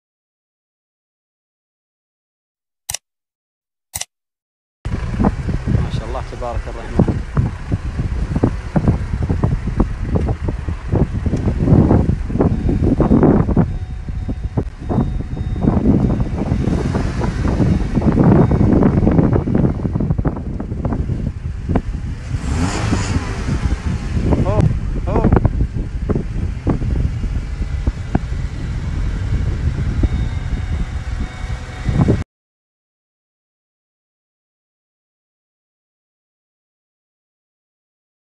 خلفه صفرا تحتها بكره للبيع التفاصيل☜رقم راعيها 0580939999
حراج على خلفه صفرا تحتها بكره
السن / سدس
السوم / 13
سليمه من جميع العذاريب
الموقع - القصيم - عنيزه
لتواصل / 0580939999